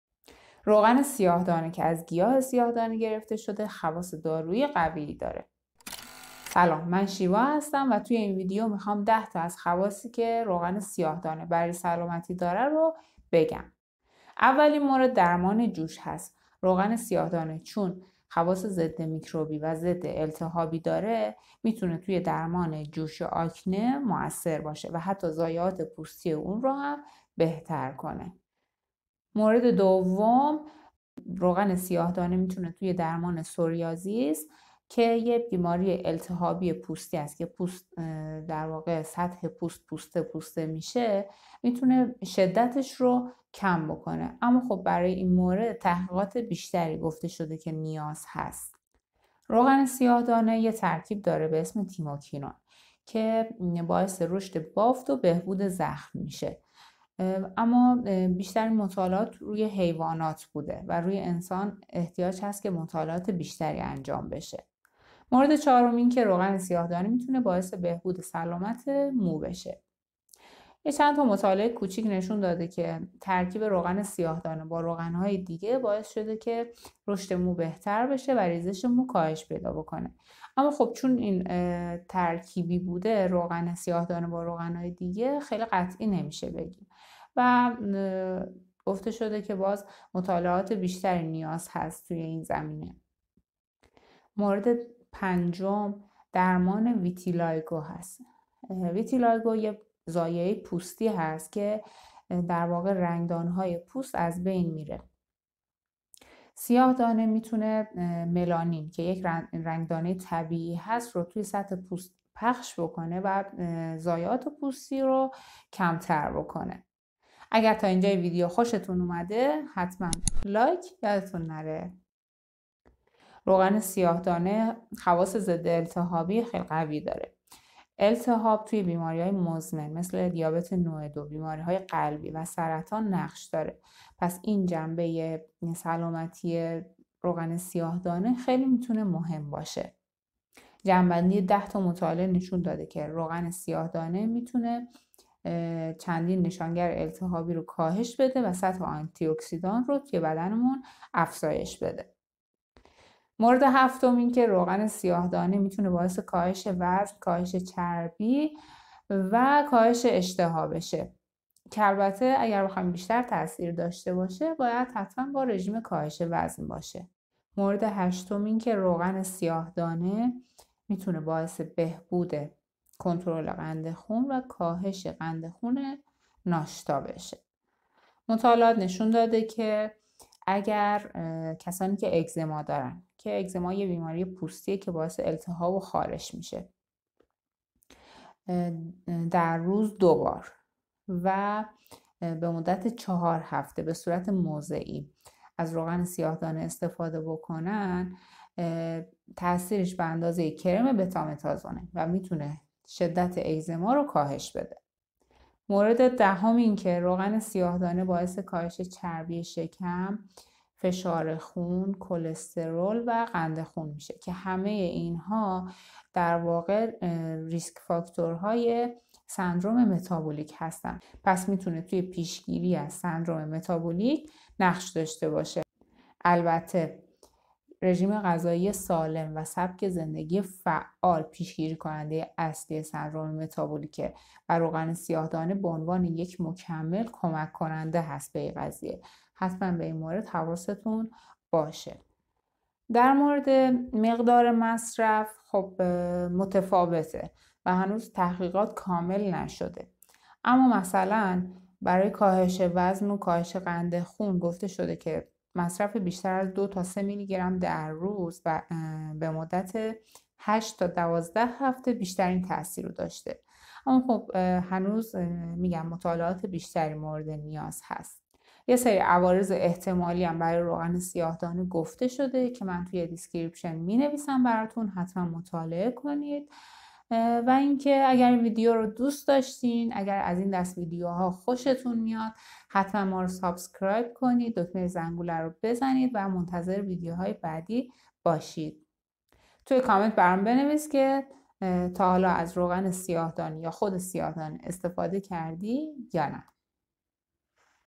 خواص روغن سیاهدانه، معجزه سیاه دانه، درمان بیماری با سیاه دانه
ویدیو جدید در مورد سیاهدانه هست که مطالب تکمیلی هم در اون گفتم. لینکش رو میذارم: